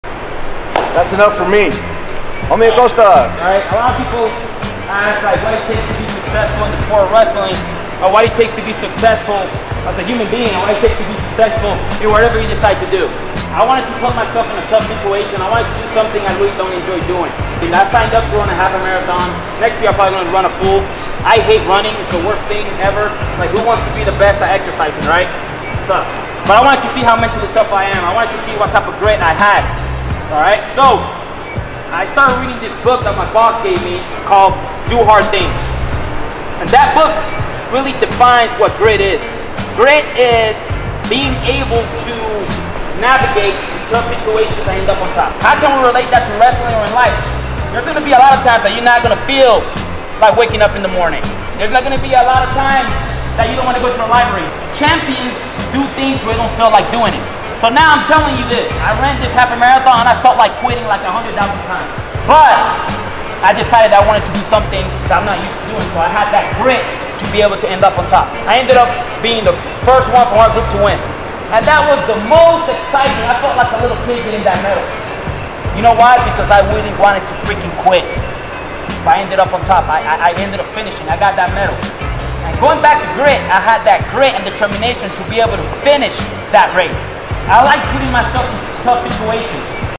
Mayfield High School Wrestling Camp 2023
Head Coach Omi Acosta and Head Assistant Cole Manion head out to Las Cruces, New Mexico for their annual wrestling camp at Mayfield High School